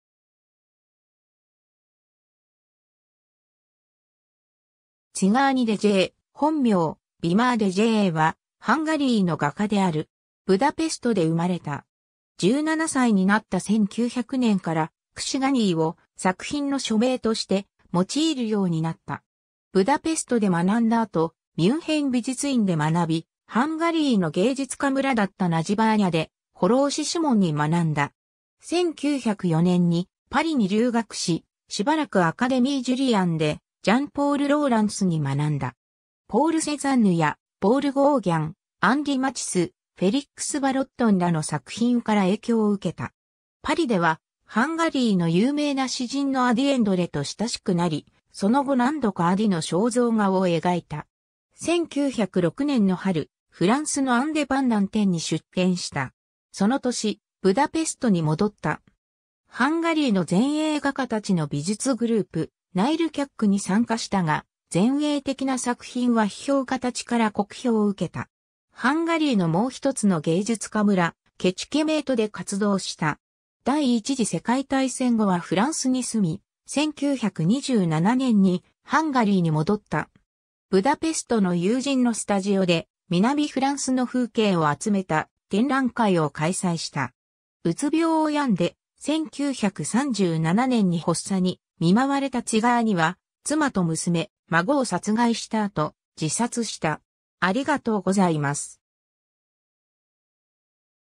ツィガーニ・デジェー
ツィガーニ・デジェー、本名、ヴィマー・デジェー（Czigány Dezső、本名: Wimmer Dezső 、1883年6月1日 - 1937年12月31日）はハンガリーの画家である。
 ブダペストで生まれた。
17歳になった1900年から、Czigányを作品の署名として用いるようになった(ハンガリー語の "czigány"はジプシーの意)。
ブダペストで学んだ後、ミュンヘン美術院で学び、ハンガリーの芸術家村だったナジバーニャ(Nagybánya:現バヤ・マレ)で ホローシ・シモン(Hollósy Simon: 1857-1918)に学んだ。
1904年にパリに留学し、しばらくアカデミー・ジュリアンでジャン＝ポール・ローランスに学んだ。
ポール・セザンヌやポール・ゴーギャン、アンリ・マチス、フェリックス・ヴァロットンらの作品から影響を受けた。
 パリでは、ハンガリーの有名な詩人のアディ・エンドレと親しくなり、その後何度かアディの肖像画を描いた 。
1906年の春、フランスのアンデパンダン展に出展した。
その年、ブダペストに戻った。
ハンガリーの前衛画家たちの美術グループ、「 Nyolcak(英語訳:the Eight)」に参加したが、前衛的な作品は批評家たちから酷評を受けた。
ハンガリーのもうひとつの芸術家村、ケチケメートで活動した。
 第一次世界大戦後はフランスに住み、1927年にハンガリーに戻った。
ブダペストの友人のスタジオで、南フランスの風景を集めた展覧会を開催した。
 うつ病を病んで、1937年に発作に見舞われたツィガーニは、妻と娘、孫を殺害した後、自殺した。